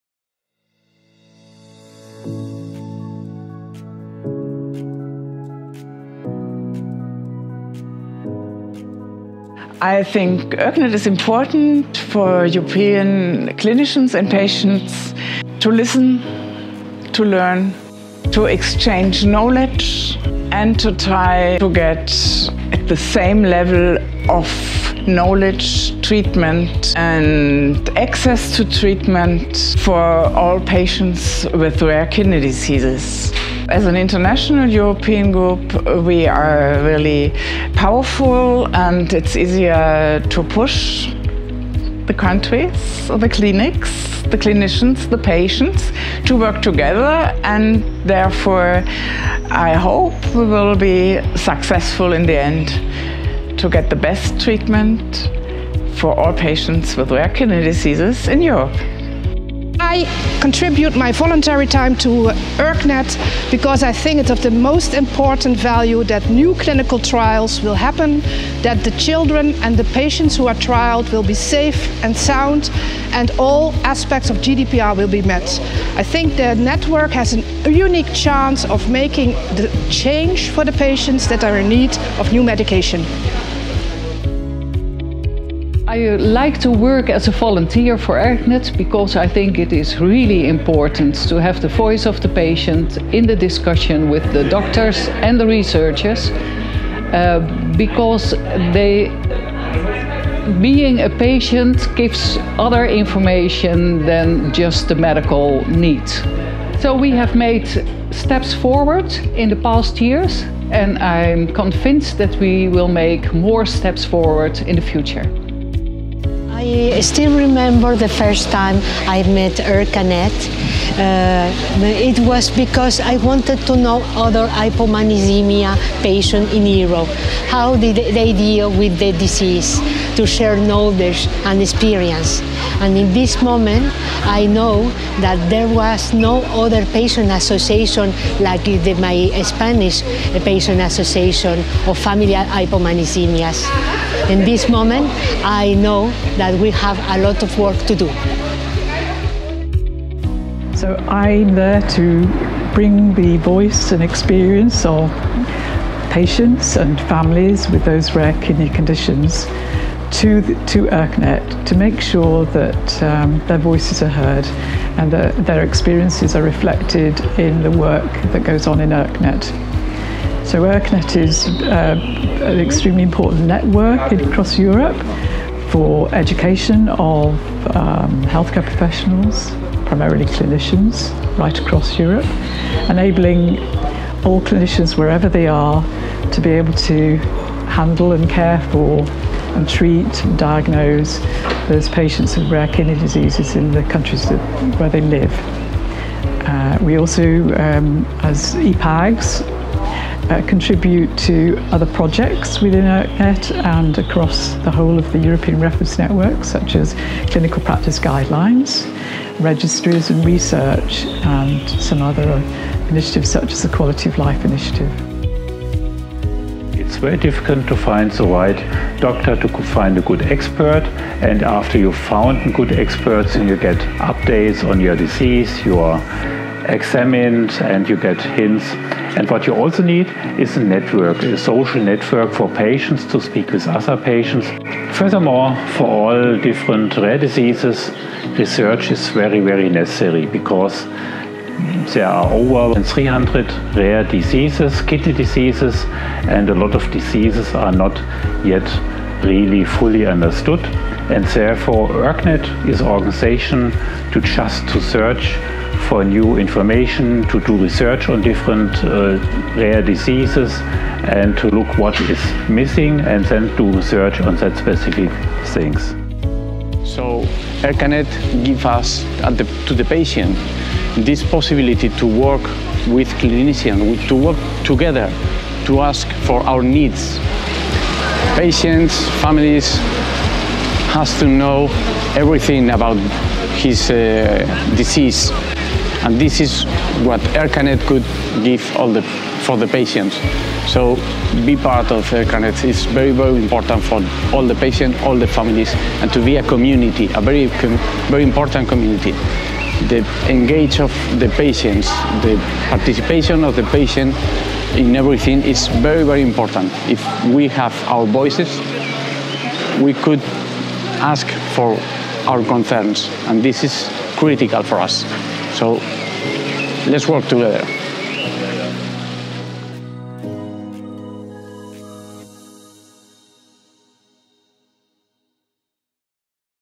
Meet the ERKNet patient representives (ePAG) (EN/EN Subs)
European Patient Advocacy Group (ePAG)
Dedicated rare kidney disease patient advocates work closely with clinicians, researchers and other healthcare professionals in the European Rare Kidney Disease Reference Network (ERKNet), consisting of more than 72 European healthcare providers and can ensure that the patient voice is heard within the network.

For more information about ERKNet and our ePAGs please visit the ERKNet patients